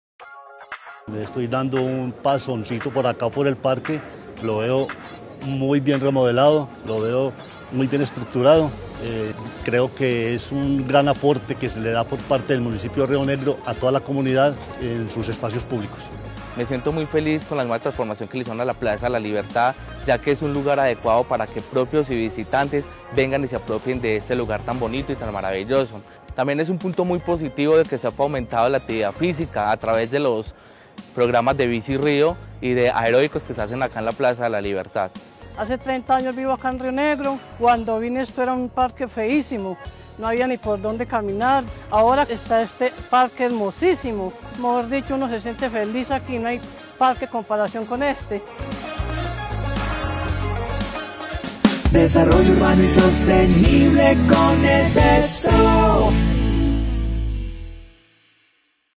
Entregamos mejores espacios públicos a los rionegreros
De la mano de Alcaldía de Rionegro, trabajamos por mejorar y multiplicar los espacios públicos para el disfrute de los rionegreros.